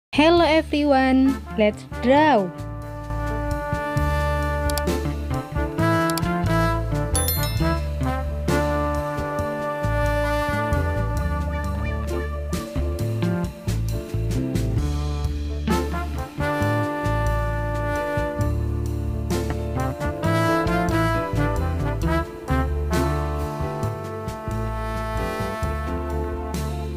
Mewarnai Lukisan Pemandangan Indah di Desa
Relax by drawing and coloring your creations.
Theme: "Beautiful Views"

Don't forget to SUBSRIBE. Thank you!